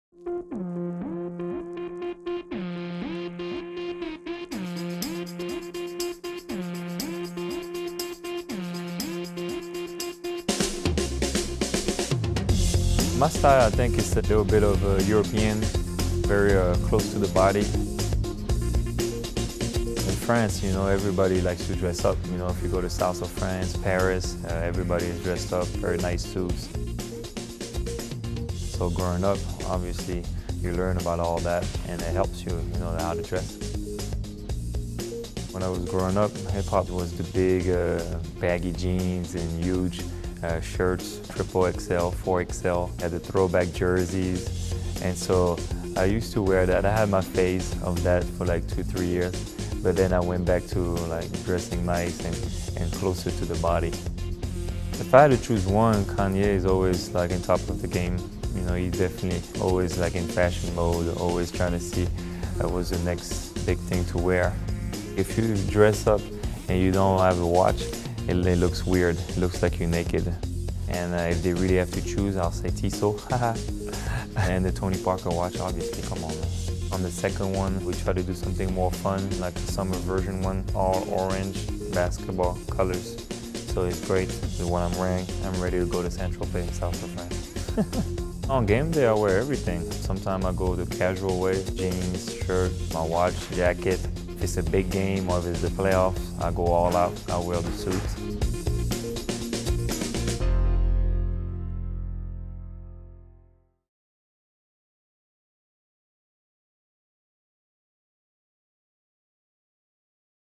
Behind the scenes footage of Tony Parker's Maxim magazine cover shoot!
Behind the scenes footage of Tony Parker's Maxim magazine cover shoot! Courtesy of Tissot. Link to post